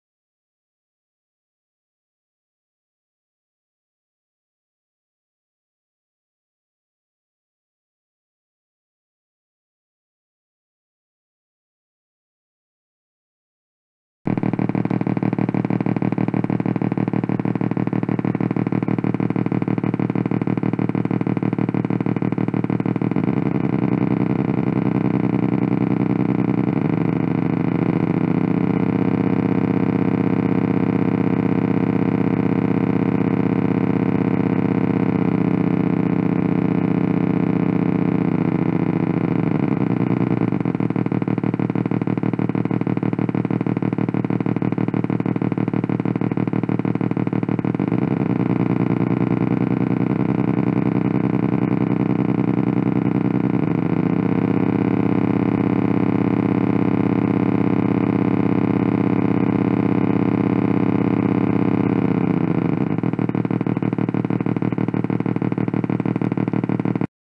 Bruiteur Diesel
J'ai réalisé le bruiteur diesel "Noiseboat" dont la description se trouve sur Bateau Modèle.